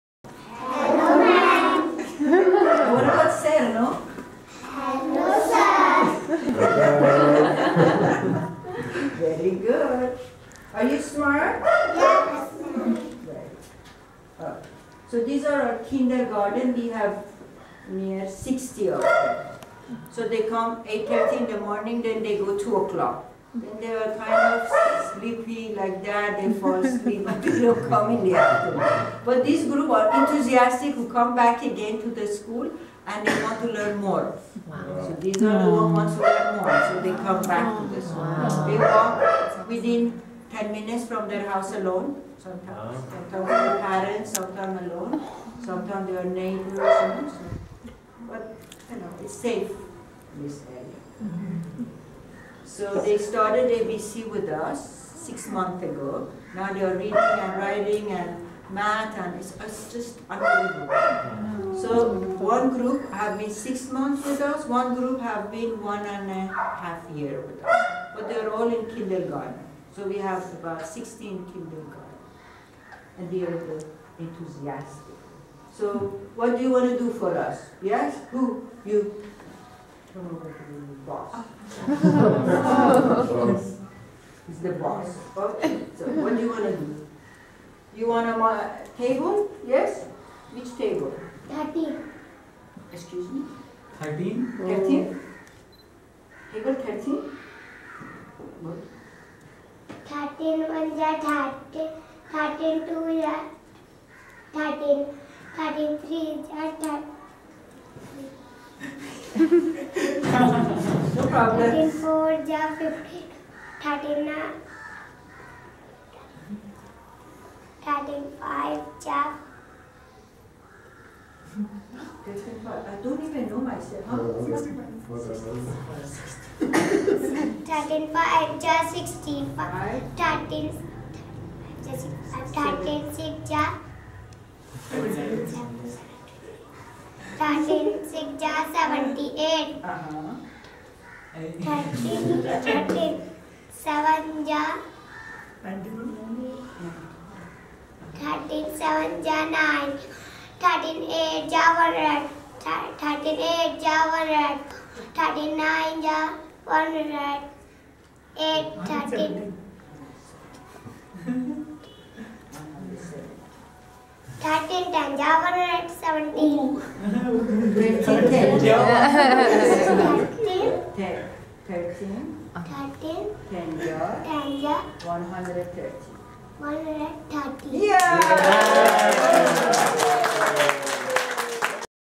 Tourist in Northern India: Visiting Mother Miracle School
Scene from the kindergarten after school program at Mother Miracle School in downtown Rishikesh.